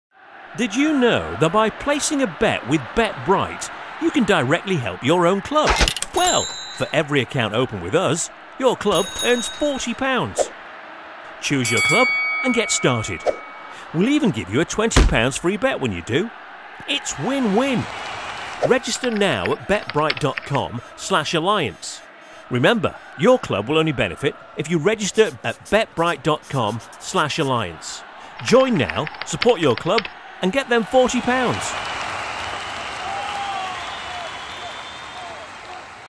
BetBright.com Alliance League Promotion
Online bookmaker BetBright.com have launched an exciting new fundraising campaign to support clubs in the Alliance League.

This video illustrates how to get involved in this innovative new scheme which football clubs and supporters alike are welcome to avail of at any time.

and register.

STEP 2: Bet £25 with us.

STEP 3: We'll give your club £40.

STEP 4: We'll give you a free £20 bet.